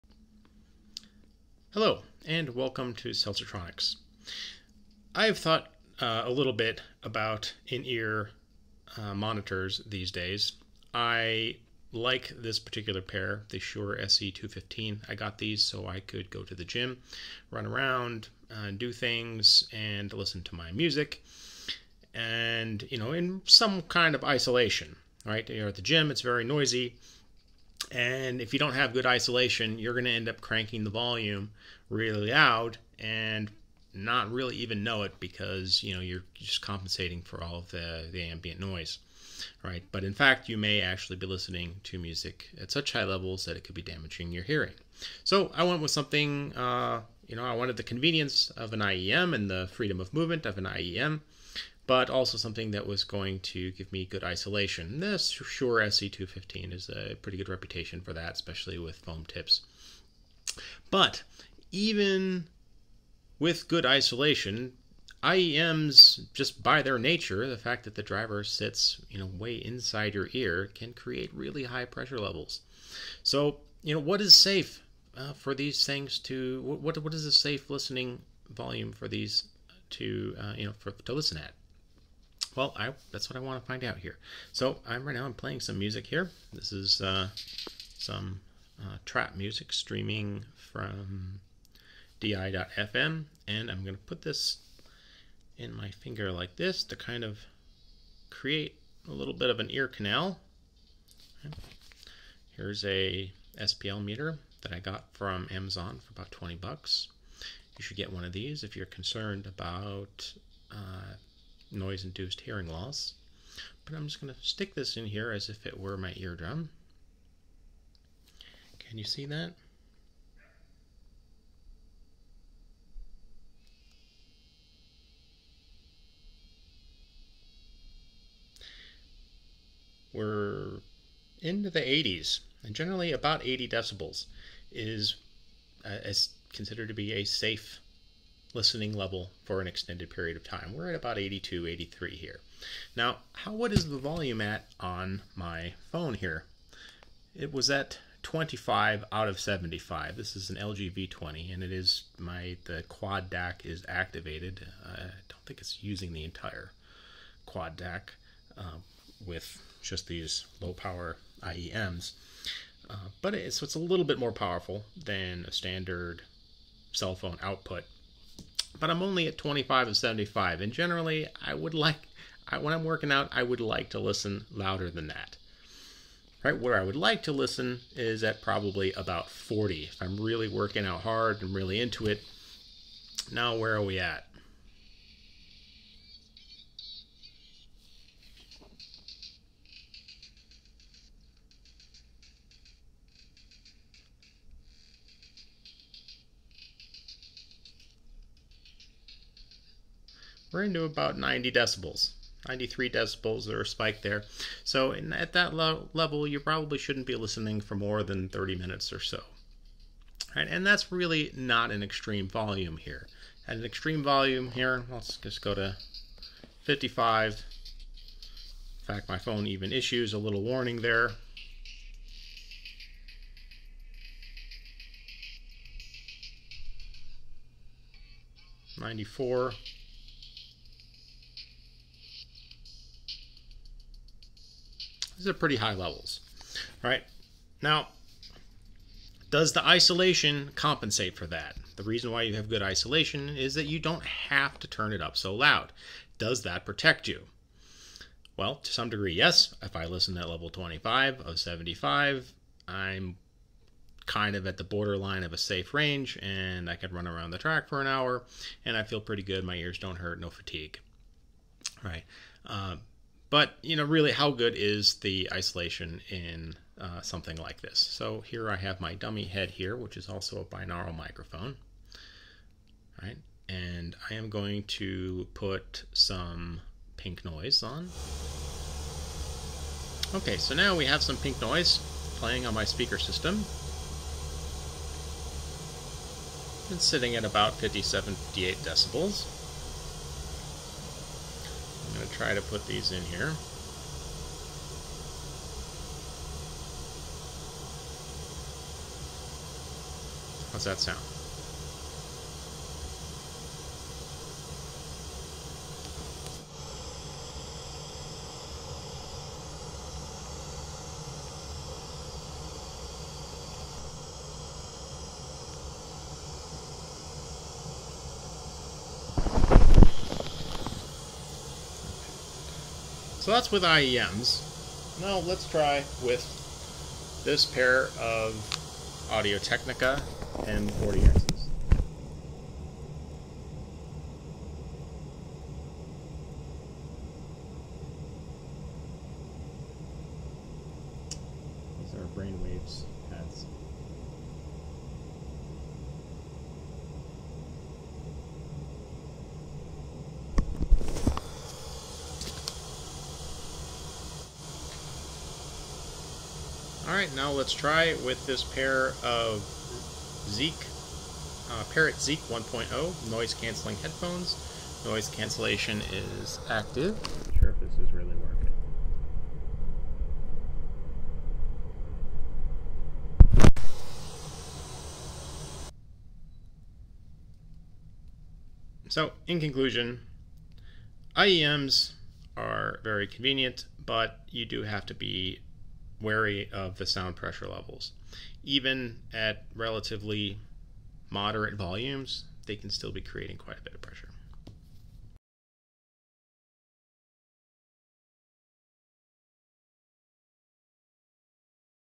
Are earbuds safe? Concerns over earbuds and sound pressure levels and protecting your ears.
My audiologist tells me she sees permanent hearing loss in kids as young as 17 due to earbuds. Protect your ears.